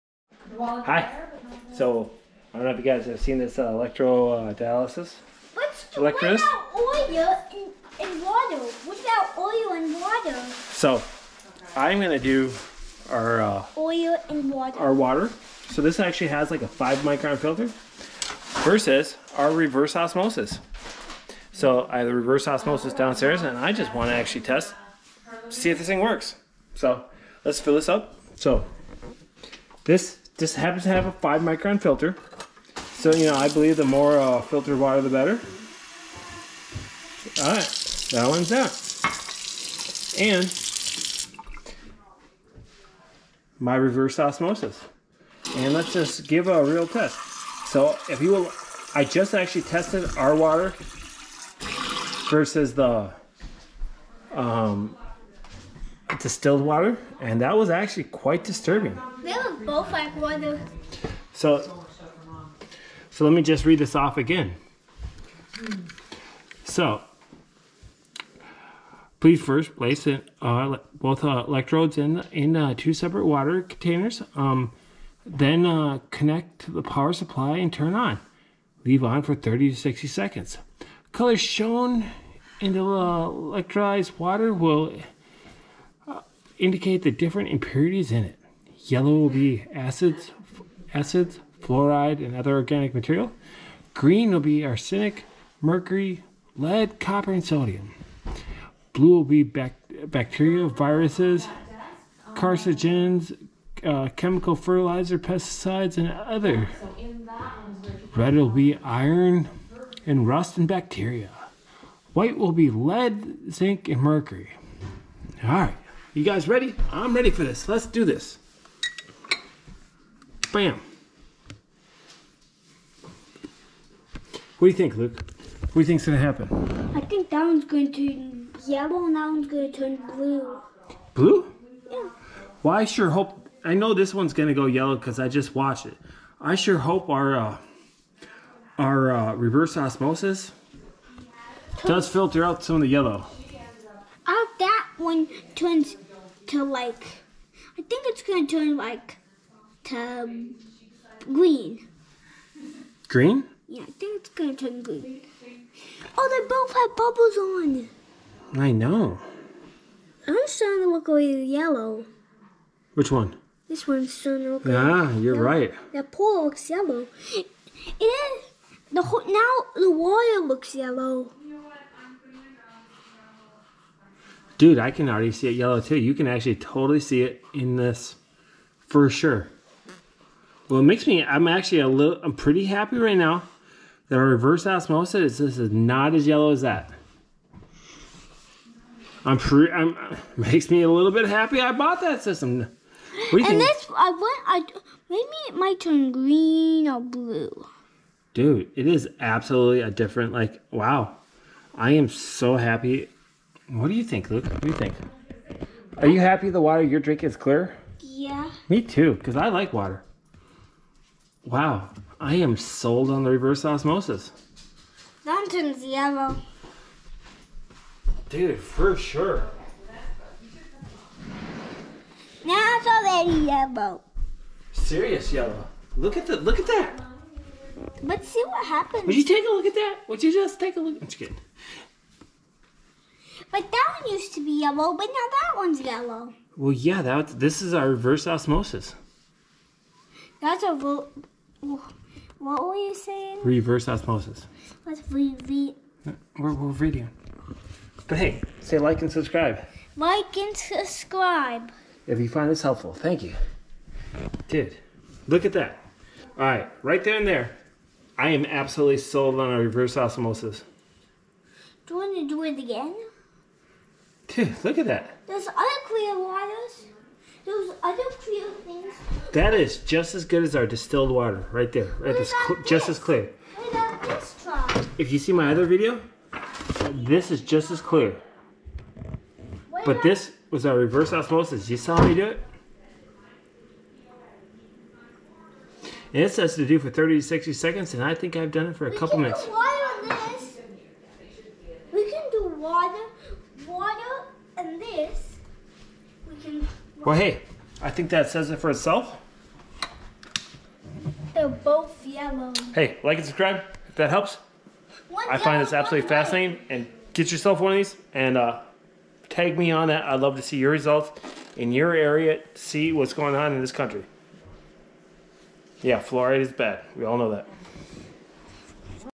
Water Testing Reverse Osmosis water against Tap water, using a water
Testing my tap water in newburgh NY against my reverse osmosis water. The results make me very happy I installed a reverse osmosis for my family drinking water...